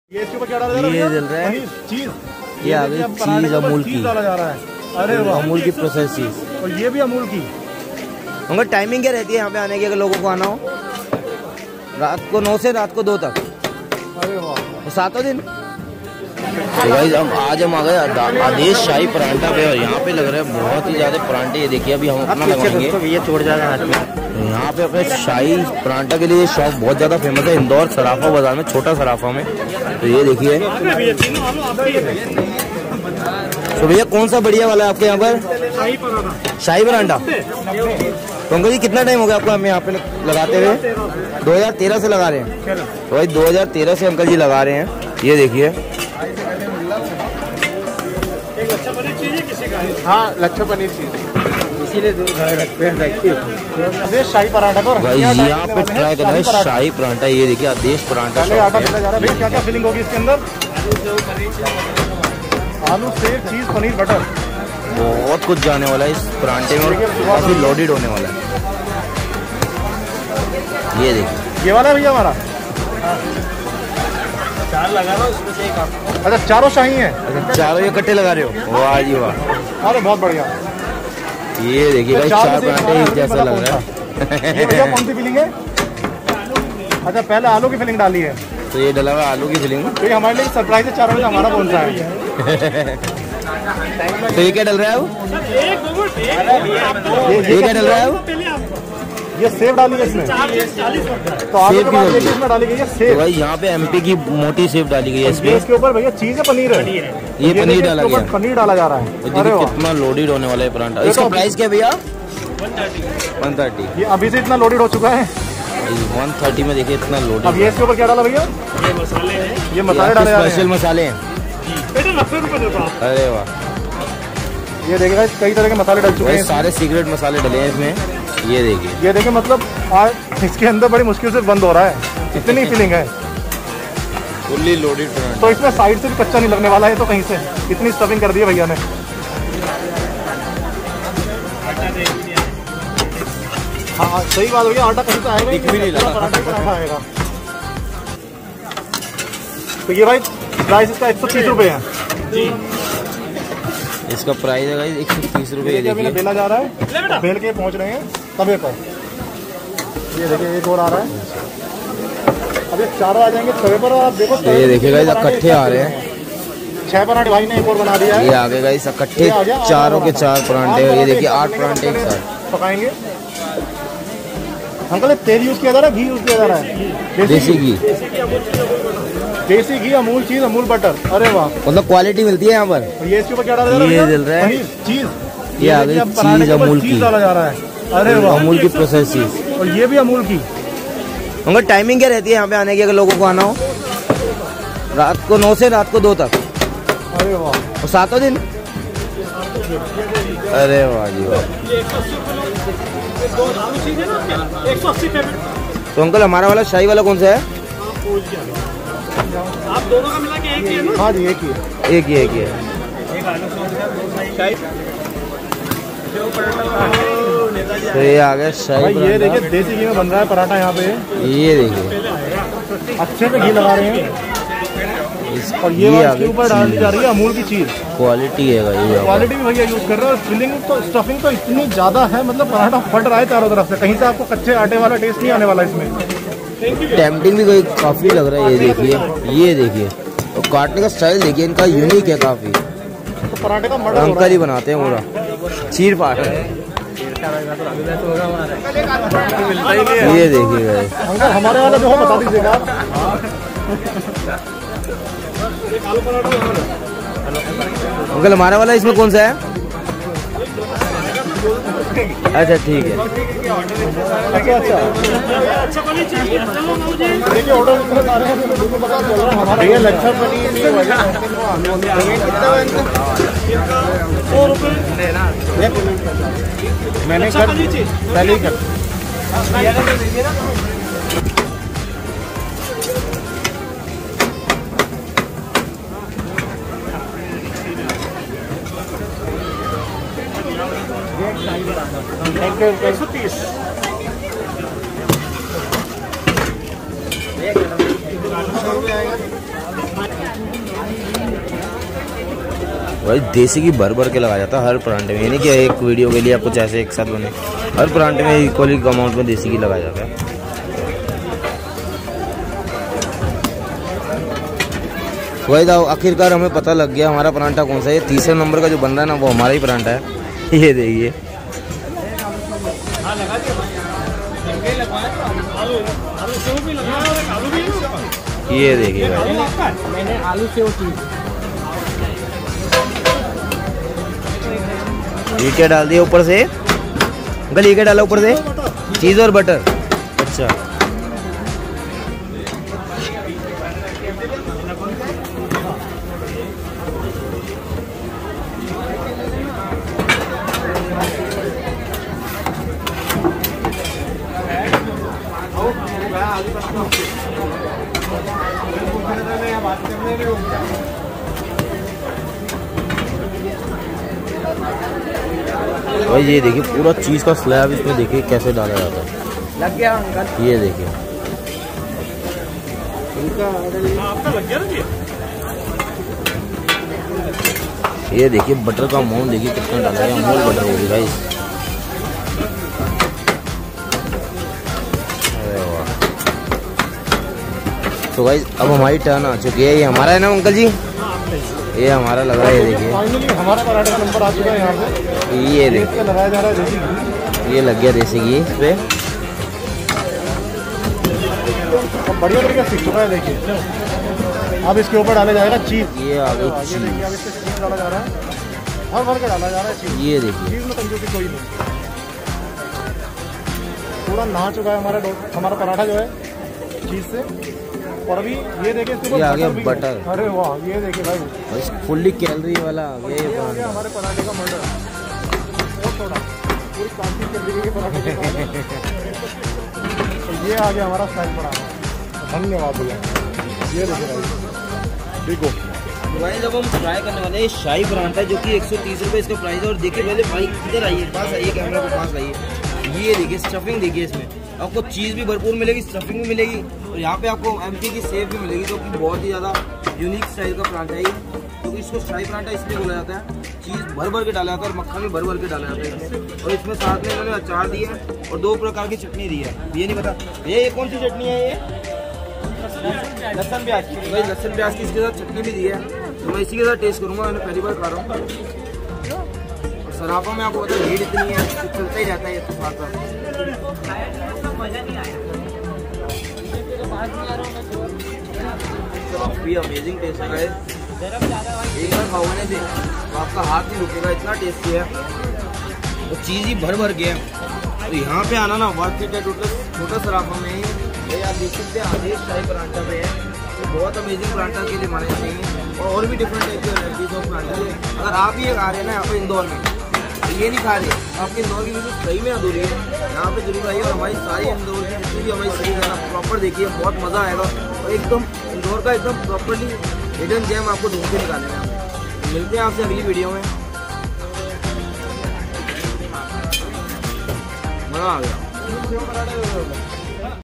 Indore's Special Shahi Parantha Made Up Of Desi Ghyo || Indian Street Food
Here you find best streetfood from all over India with honest review not fake like other bloggers does